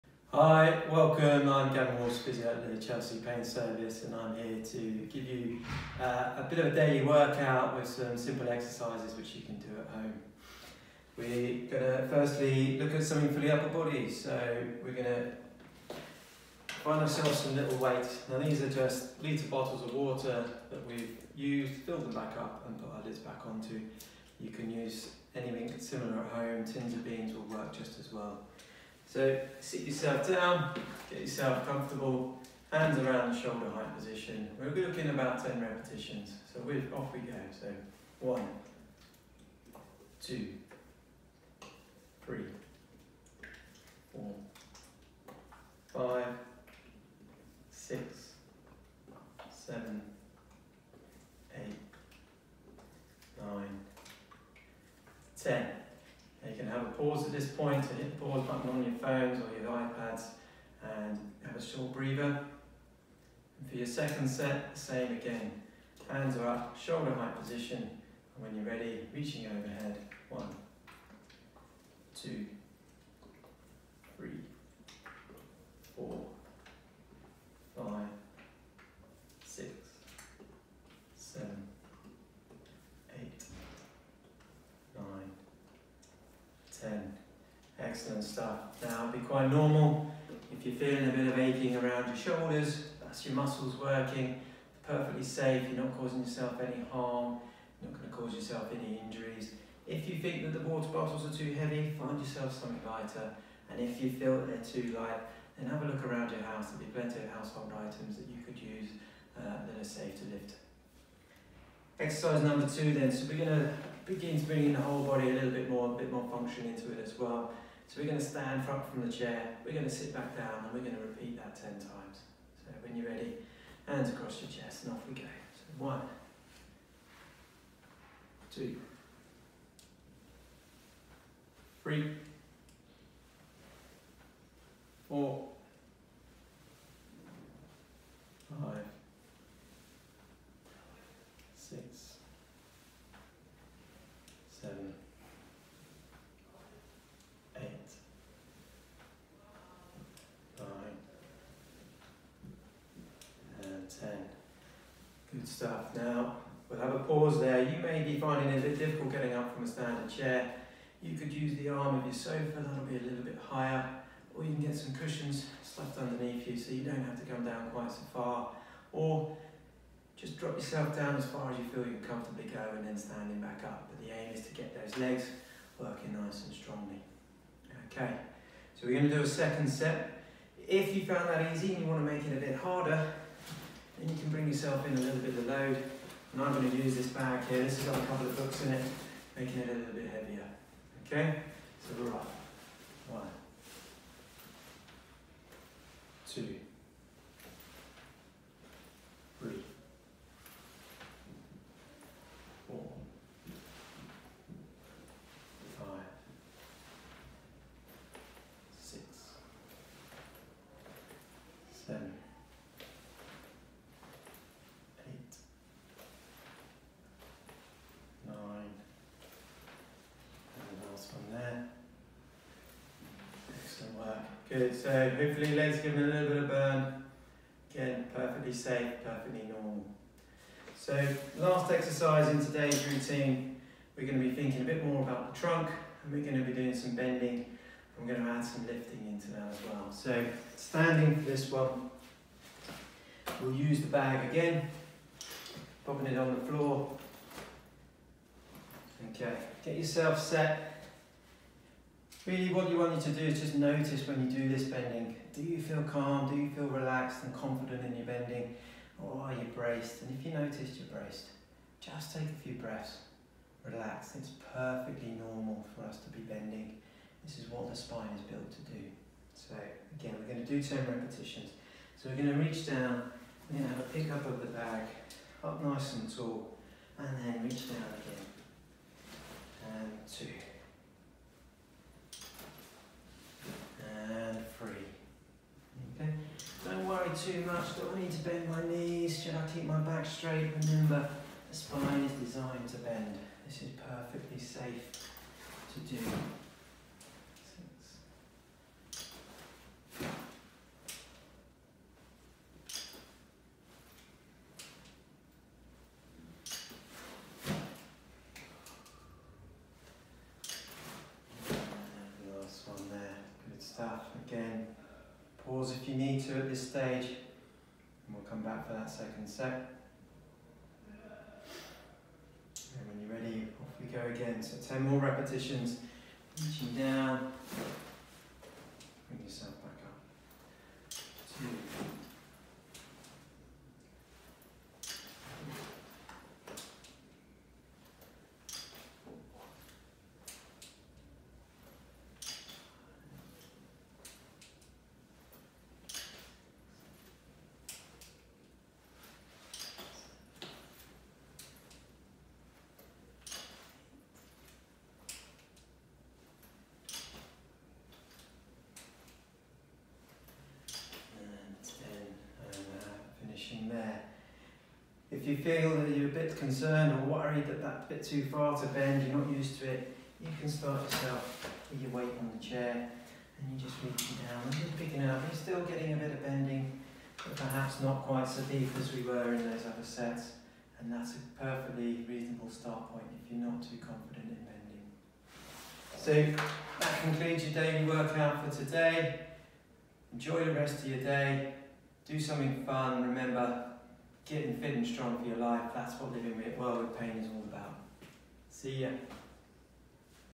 Exercise Routine - Day 1
Here is a great 5 minute exercise routine you can do at home to keep yourself moving. All of these exercises are completely safe and involve very little equipment